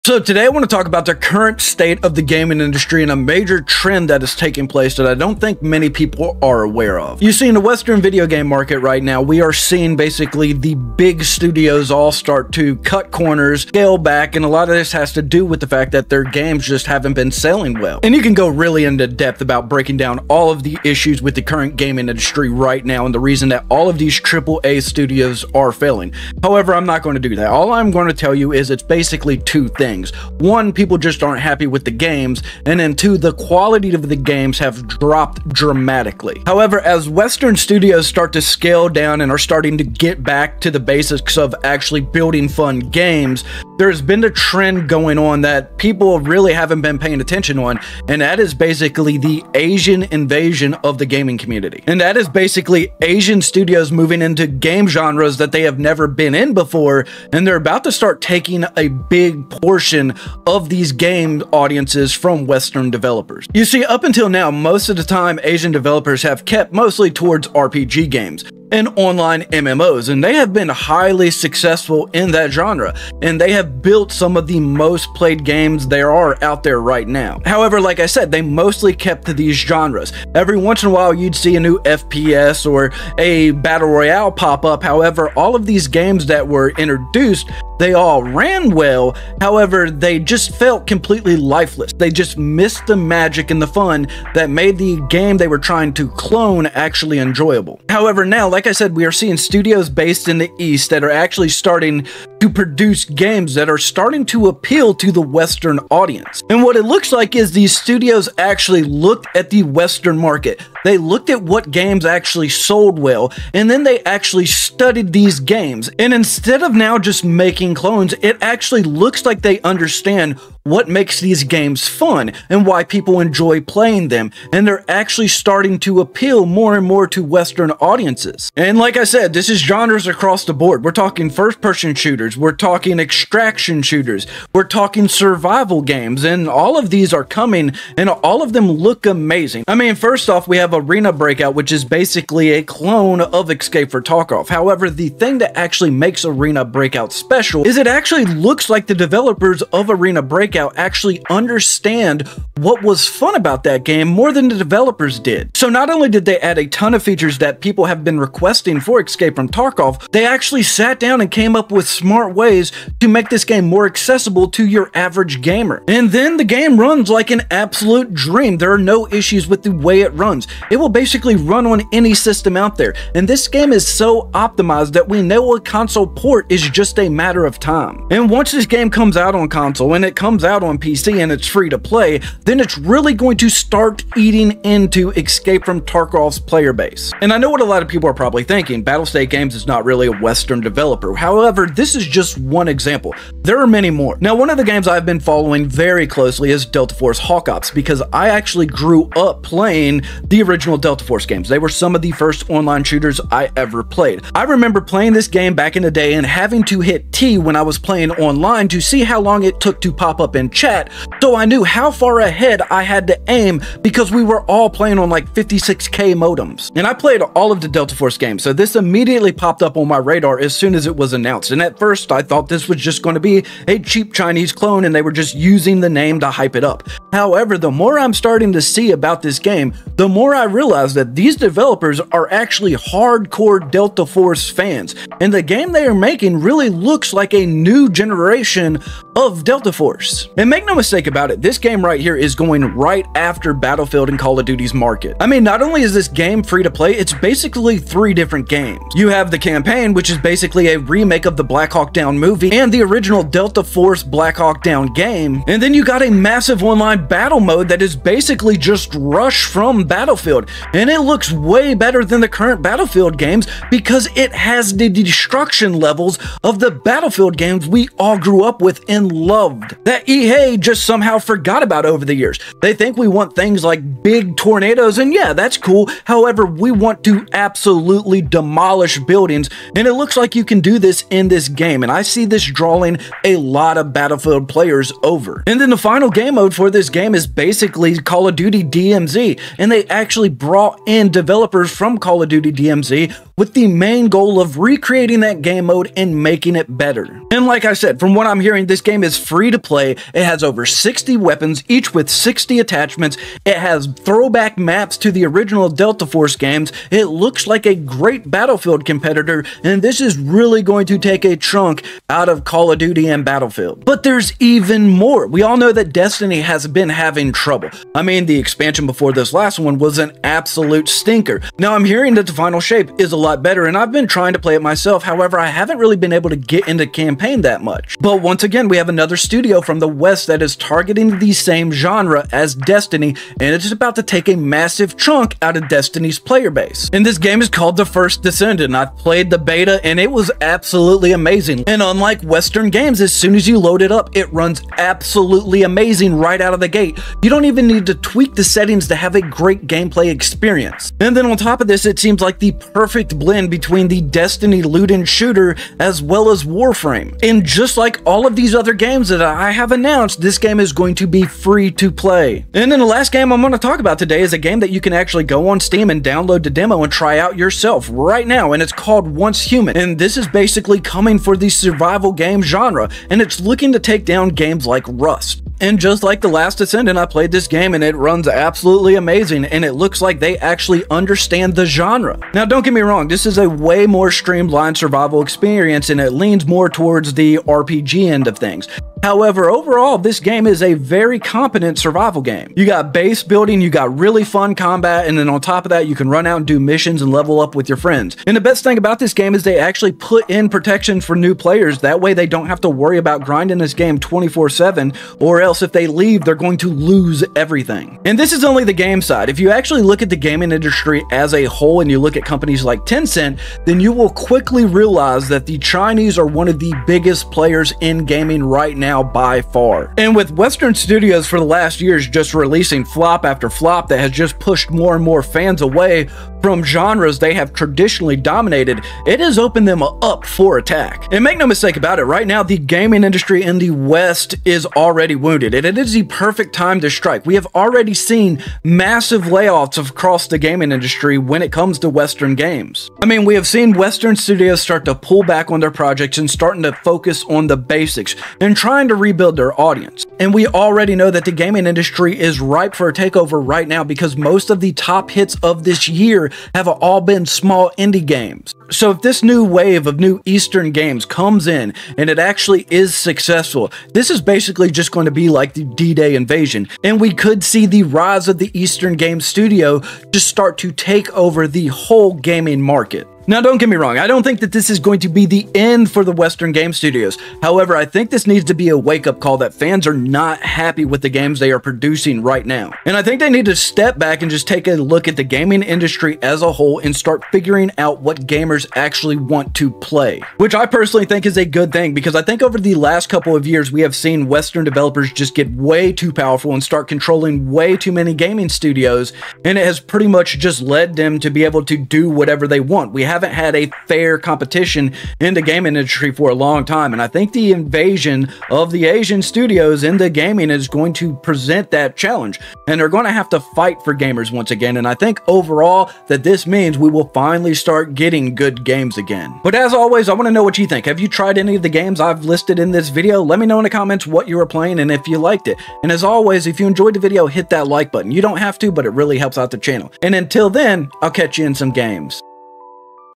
These Asian Games Will Challenge The Gaming Industry!
While Western Studios enter a period of decline, studios on the east are now on the rise and will soon be going head to head with some of the biggest franchises including, Call of Duty, Battlefield, and Destiny with new titles like Delta Force Hawk Ops, The First Descendant, and Arena Breakout Infinite.

In this video I break down my thoughts on the rise of Asian Gaming Studios in the gaming Industry and how it will effect western studios. Hope you enjoy.